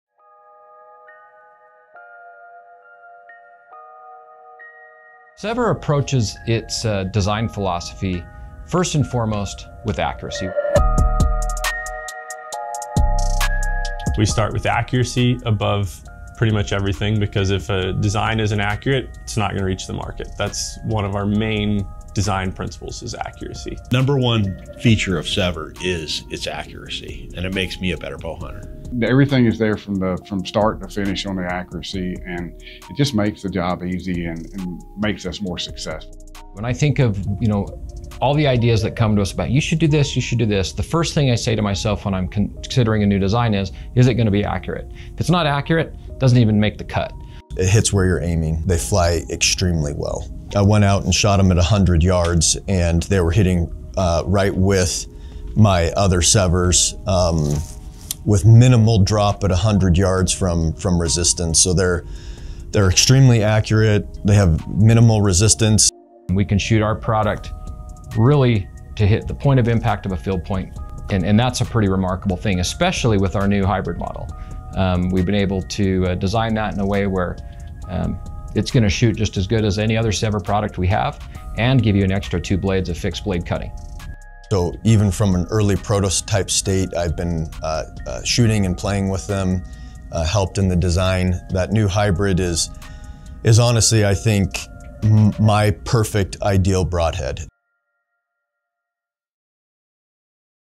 SEVR // Superior Broadhead Accuracy
SEVR’s patented, pivoting rear-deploy blade design keeps the blades fully contained during flight. The reduction in surface area offers true field-point-like flight characteristics.  For seasoned bowhunters, the advantage is clear—knowing exactly how your arrows will fly and where they will hit, leads to far greater success in the field when the big moment comes.

ADDITIONAL SEVR BROADHEAD FEATURES:

• Lock-and-Pivot™ Blades – SEVR's patented Lock-and-Pivot™technology ensures blades lock open on impact and pivot around hard-bone obstacles to maintain a straight and true arrow trajectory through various shot angles and blade-on-bone impacts. This SEVR-EXCLUSIVE innovation promotes superior penetration compared to conventional rear-deploy non-pivoting broadheads.

• Practice Lock™ Practice Mode – For practice, the exclusive Practice Lock™ allows training with actual broadheads, ensuring accurate point-of-impact verification.

• Stretch Cut™ – SEVR's Stretch Cut™ blade effect stretches the hide while cutting, resulting in a wound diameter larger than the blade itself. This feature promotes bigger blood trails and facilitates reliable recoveries.

• Custom Quantities – Sold individually in any quantity desired.